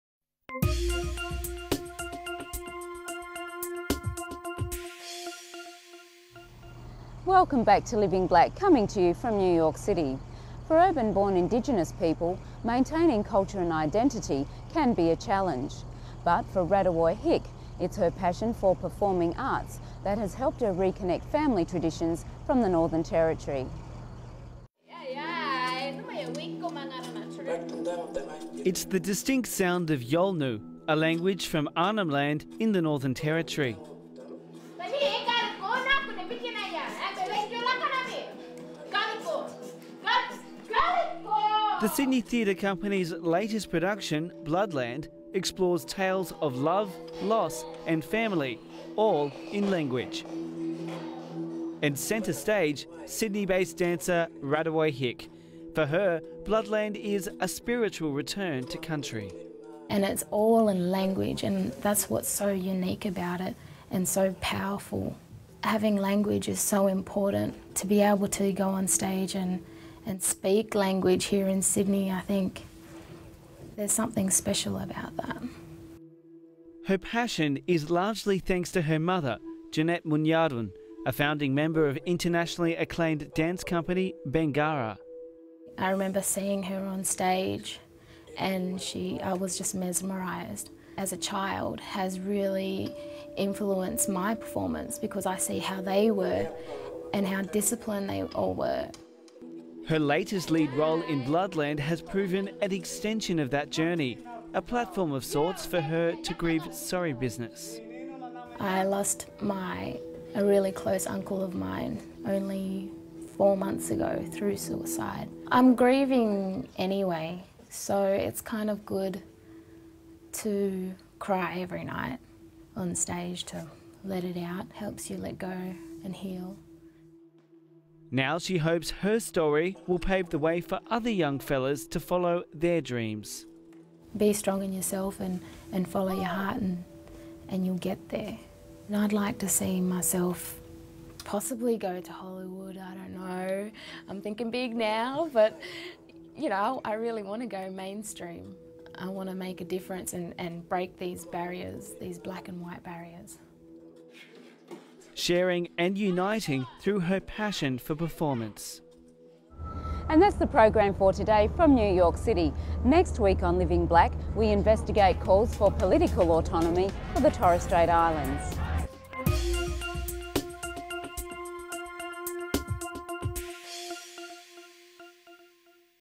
Bloodland
For urban born Indigenous people maintaining culture and identity can be a challenge.
But for Rarriwuy Hick her passion for performing arts has helped her reconnect with family traditions